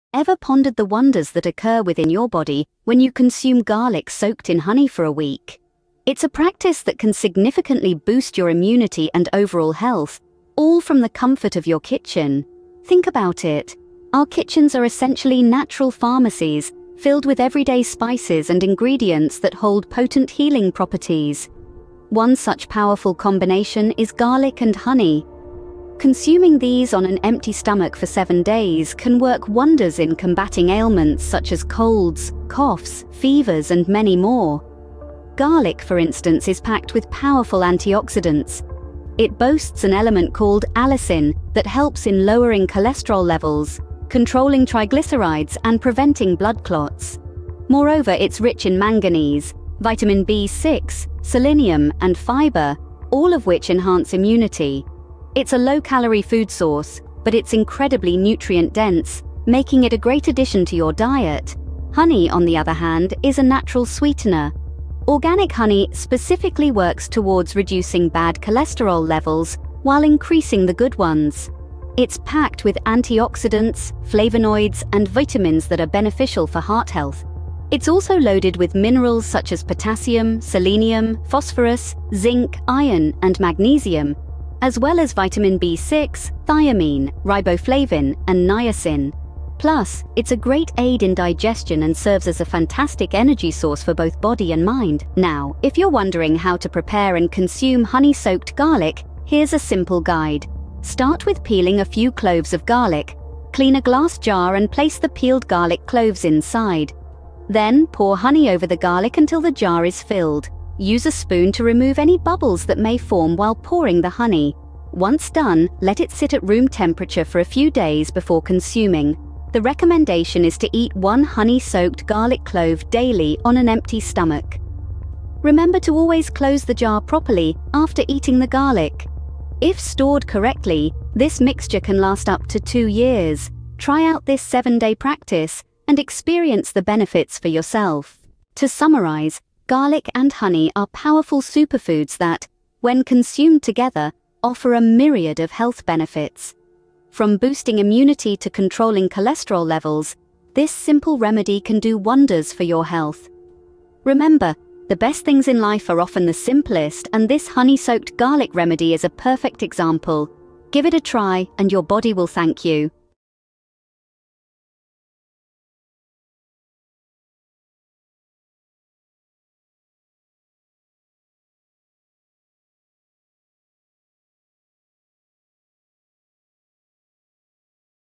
Unlocking the Healing Powers of Garlic & Honey: 7-Day Immunity Boost Challenge
Explore the miraculous health benefits of consuming garlic soaked in honey for 7 days straight! Discover how this simple remedy can enhance your immunity, fight colds, coughs, and more. Join us on a journey into nature's medicine cabinet as we delve into the science-backed advantages of incorporating garlic and honey into your daily routine. Learn the recipe, benefits, and experiences firsthand from a maternal and child nutritionist Doctor. Don't miss out on this powerful combination for a healthier you!

Garlic and honey recipe
Please take a few cloves of garlic and peel them.
Then clean a glass jar and color the garlic in it.
Finally, pour the honey over the jar filled with garlic.
Use a spoon to remove any bubbles that may form while pouring the honey.
It should be refrigerated at room temperature for a few days before eating.
Soak 1 clove of garlic in honey daily and eat it on an empty stomach.

After eating the garlic, close the jar properly. If you do this, you can eat for 2 years. You should also eat garlic soaked in honey for 7 days.